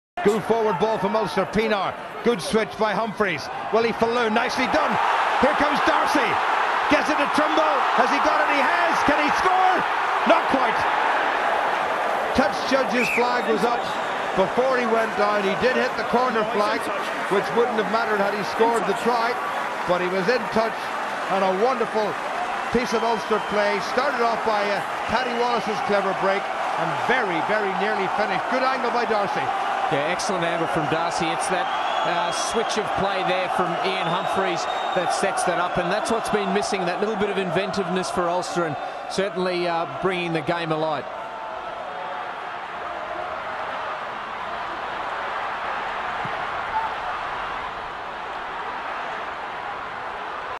Ben Morgan try-saving tackle v Ulster
Ulster v Scarlets - Magners League - Ben Morgan with a try saving tackle on Andrew Trimble